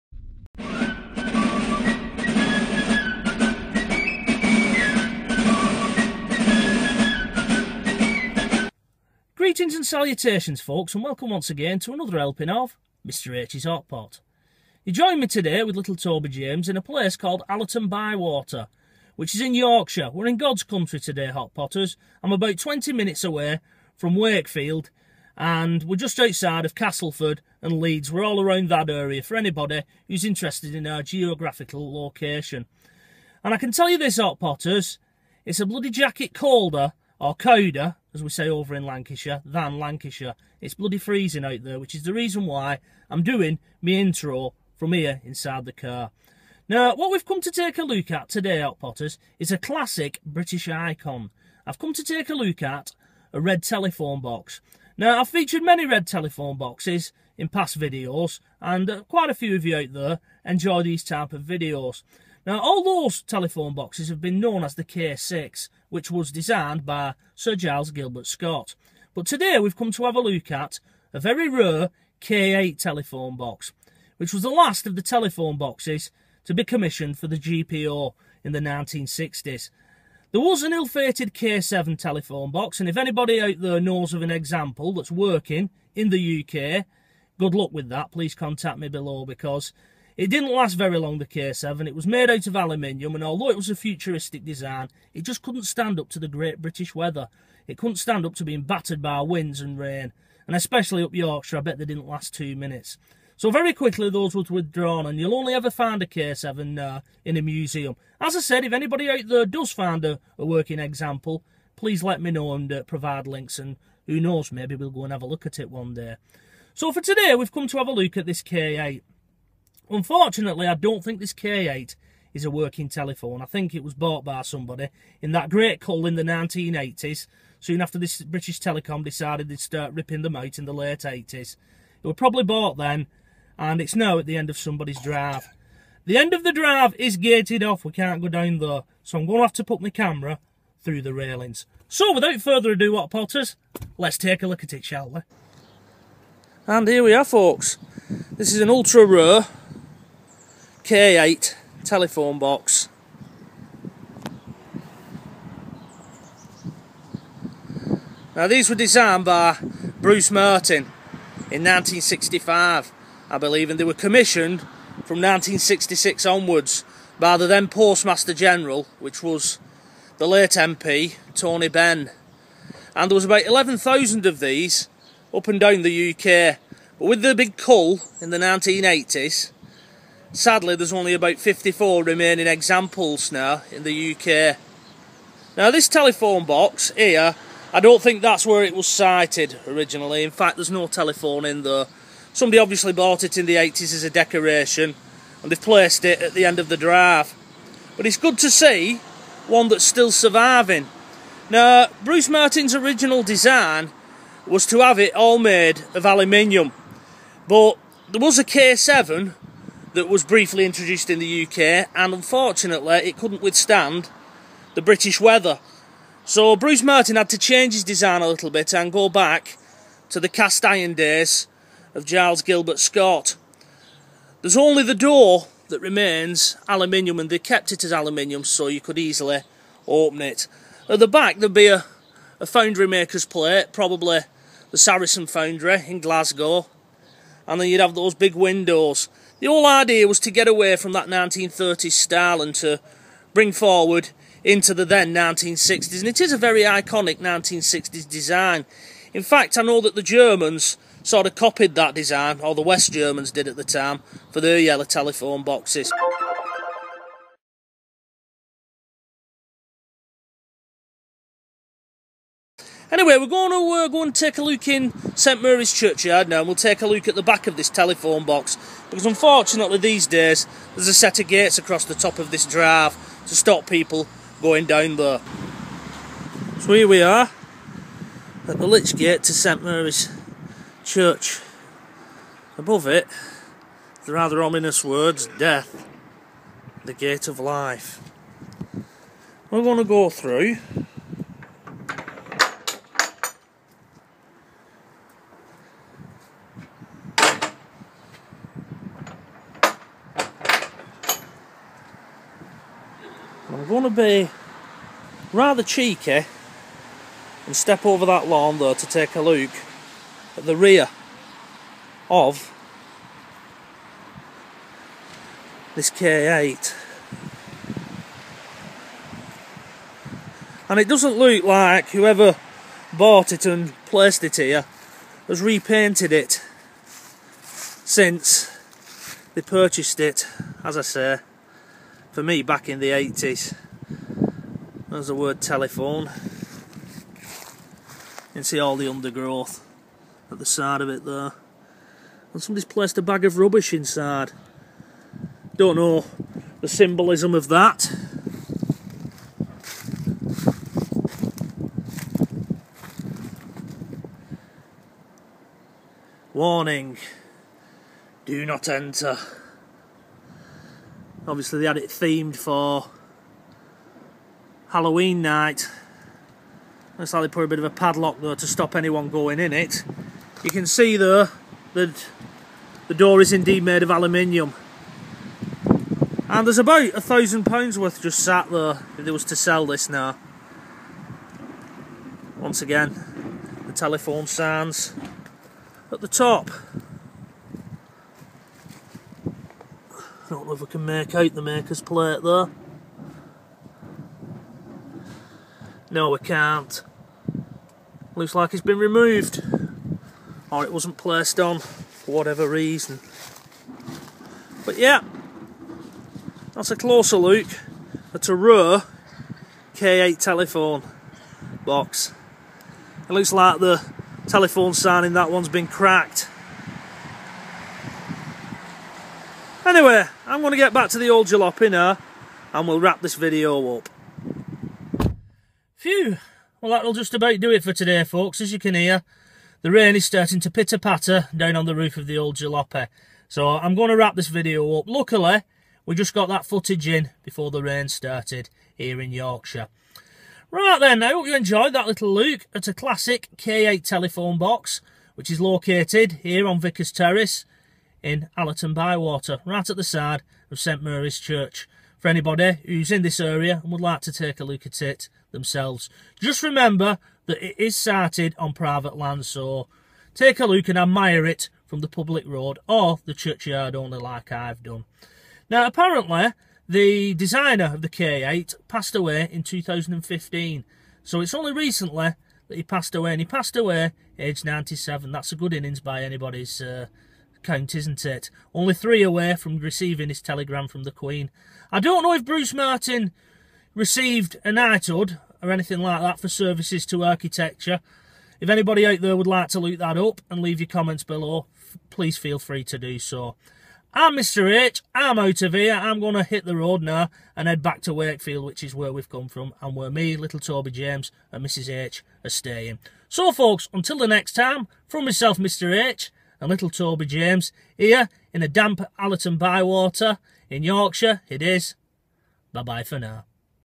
The K8, the last of the great British red telephone boxes
Mr H visits Allerton Bywater in West Yorkshire to take a look at a former red K8 telephone box which now sits as a garden ornament at the end of someone's drive but was once one of the 11,000 telephone boxes designed for the GPO by architect Bruce Martin and installed across the UK between 1968 and 1983.